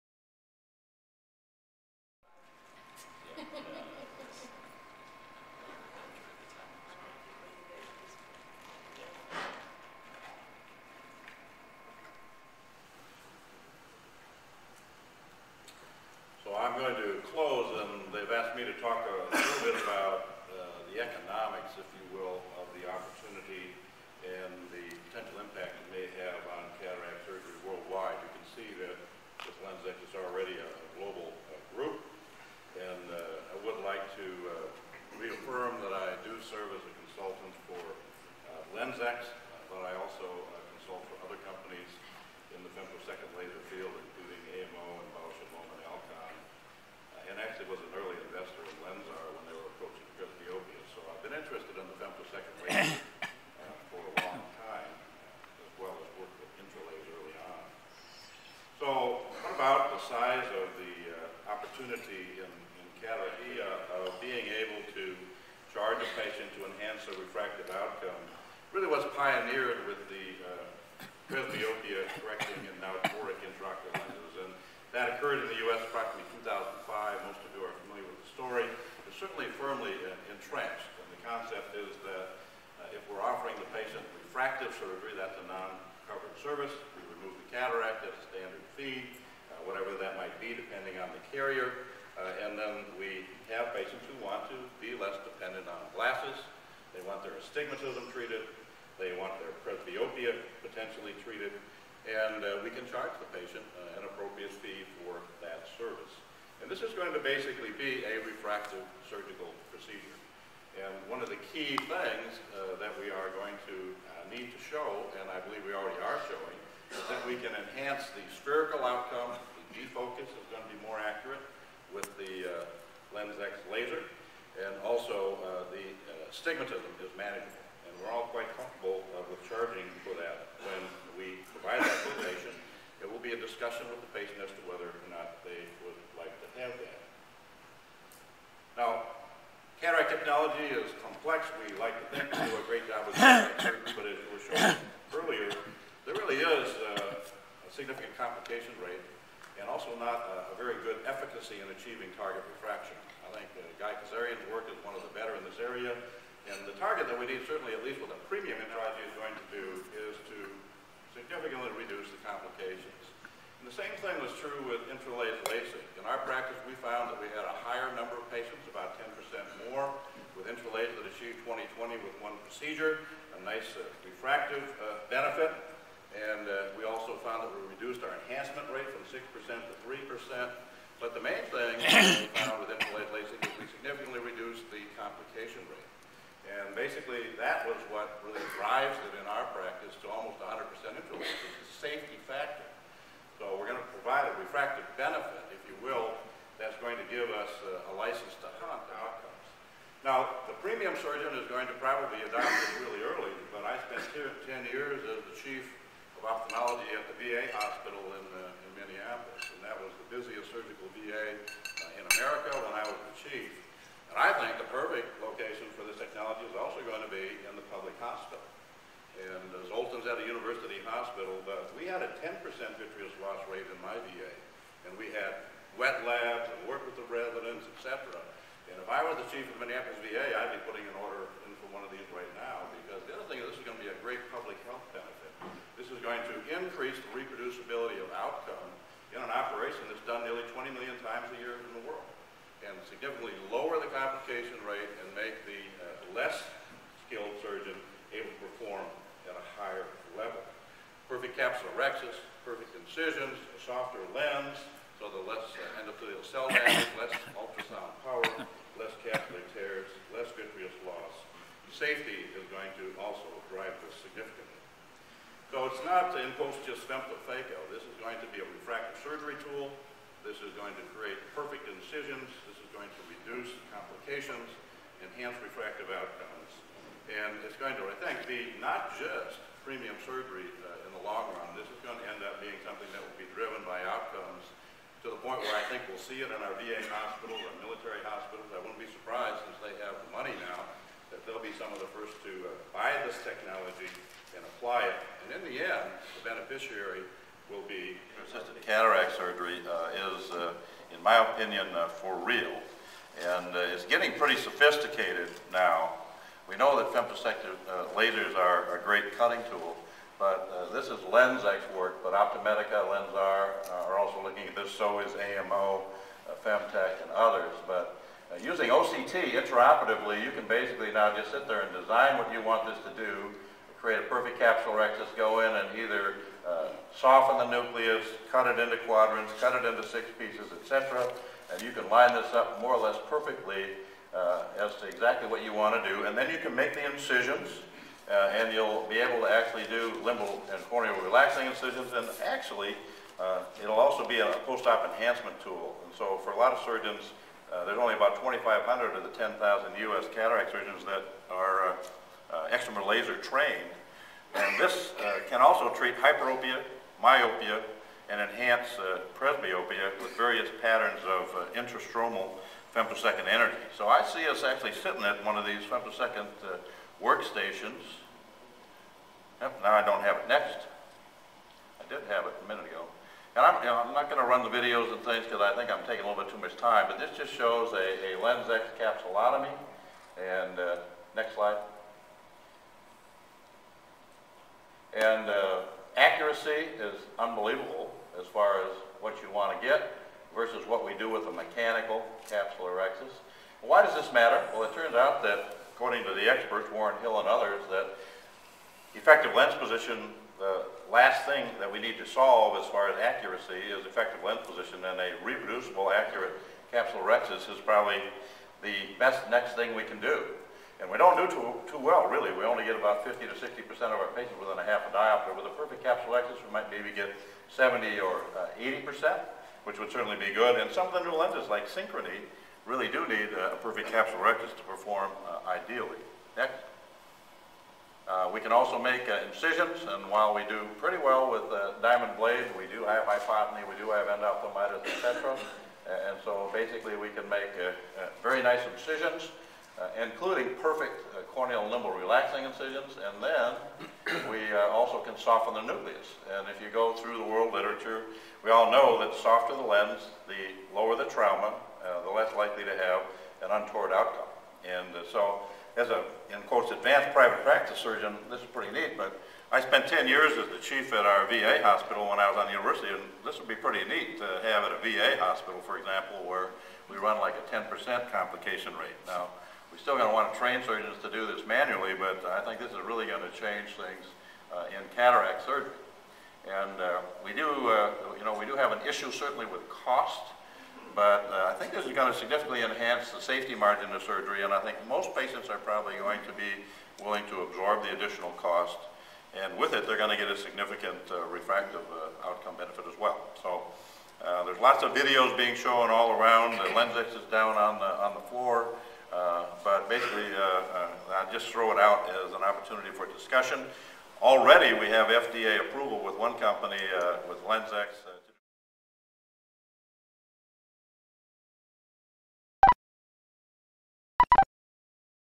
Dr. Richard Lindstrom + Femtosecond assisted cataract surger
Dr. Lindstrom describes the many advantages of using femtosecond lasers in cataract surgery.